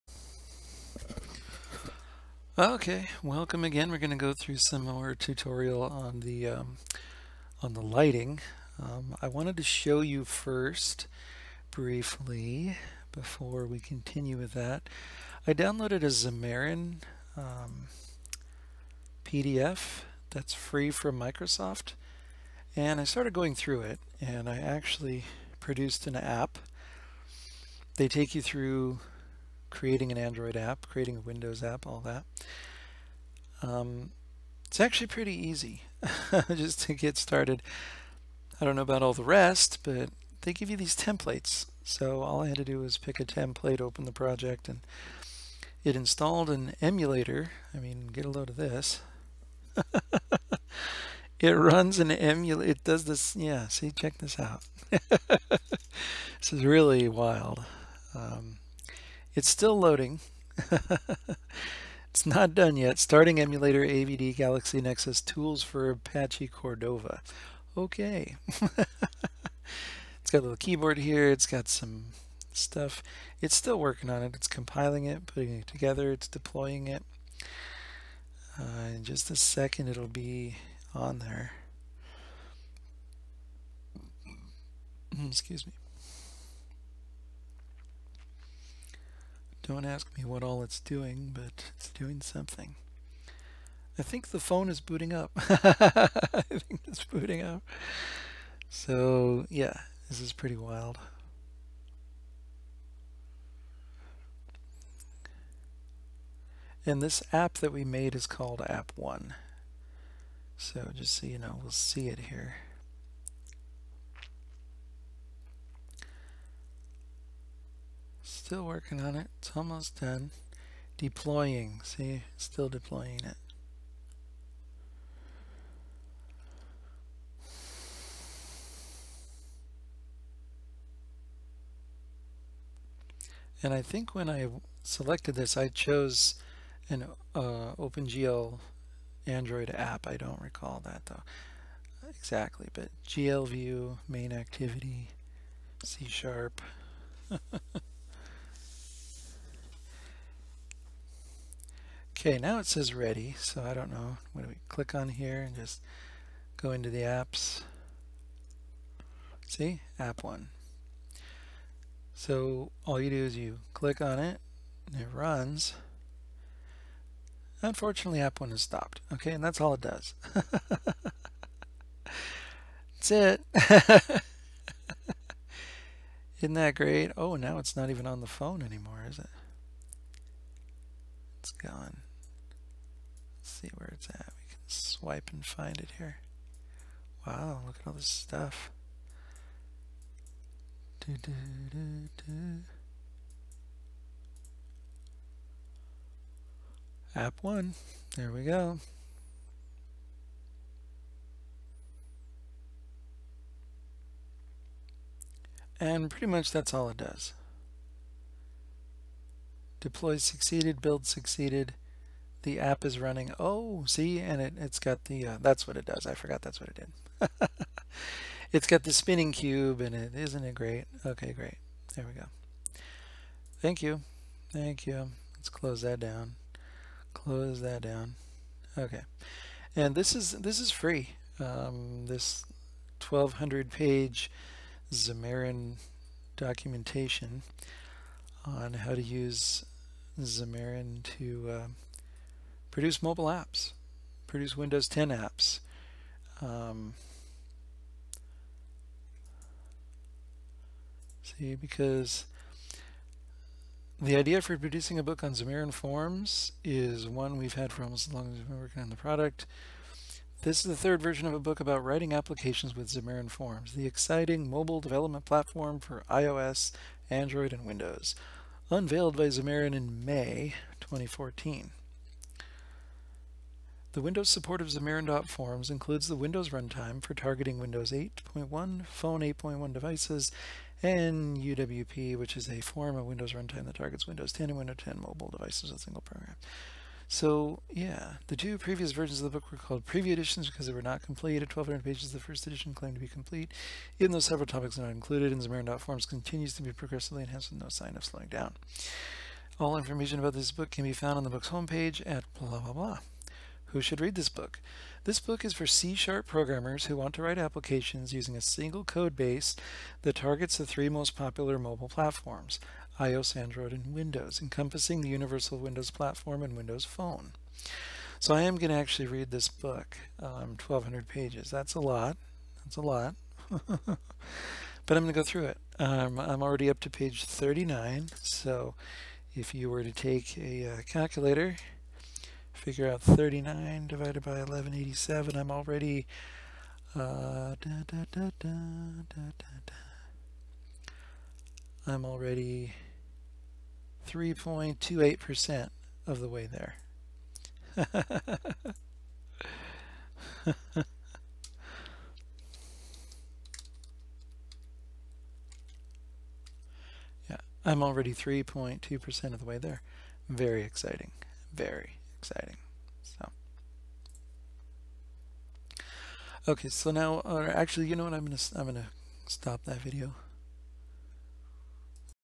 Xamarin Free from Microsoft 1200 Page PDF
This is the free documentation that I received from Microsoft recently, and I have read 3.3% of the book, and already created my first app, you can see the results in this video.